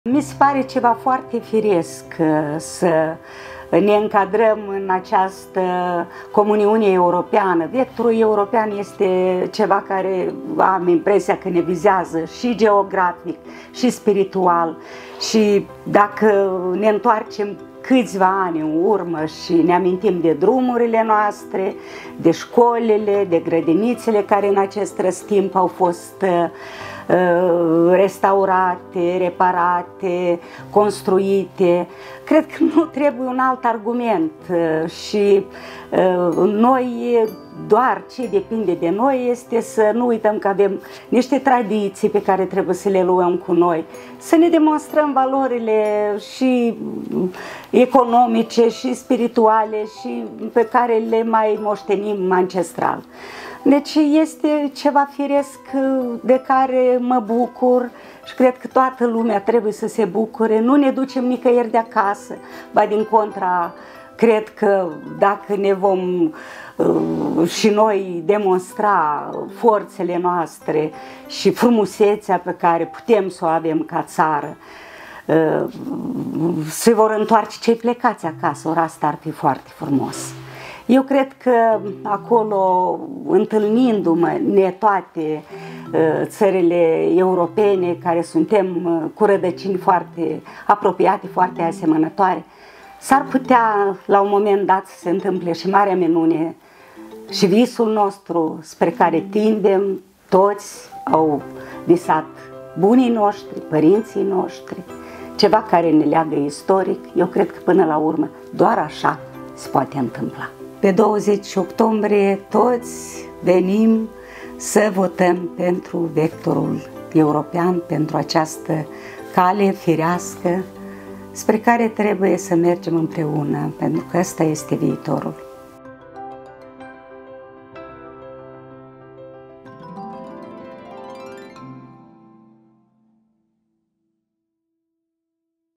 Claudia Patrole: „Pe 20 octombrie, toți venim să votăm pentru vectorul european.” 🇲🇩🇪🇺
„S-ar putea să se întâmple marea minune și visul nostru spre care tindem cu toții. 🇲🇩🇪🇺 Pe 20 octombrie, toți venim să votăm pentru vectorul european, pentru această cale firească spre care trebuie să mergem împreună, pentru că acesta este viitorul.” Claudia Patrole, poetă, eseistă și prozatoare din Republica Moldova.